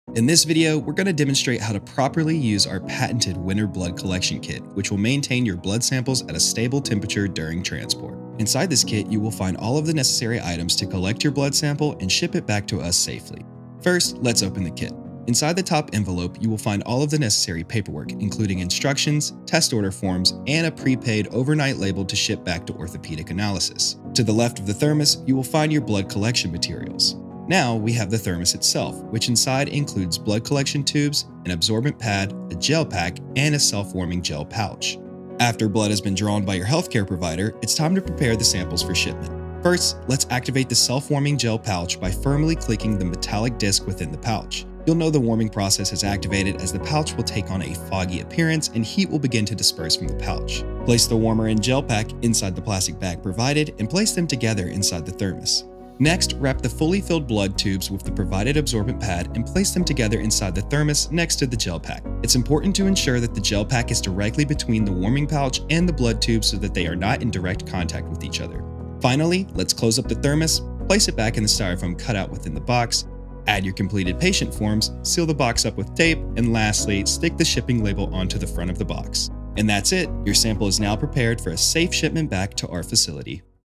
Orthopedic Analysis Winter Blood Collection Kit for Live Blood Cells Transport (Metal LTT)
This video demonstrates how to properly and safely pack your blood sample using our patented Winter Blood Collection Kit.
Orthopedic Analysis has designed and patented a blood collection and transport kit that will maintain your blood sample at a stable temperature  during shipping overnight back to our facility for testing (Lymphocyte Transformation Test LTT). It is imperative that blood samples arriving at our facility contain live blood cells in order to yield accurate results. The use of our kit will ensure that your sample is secure, safe and viable for testing once it arrives.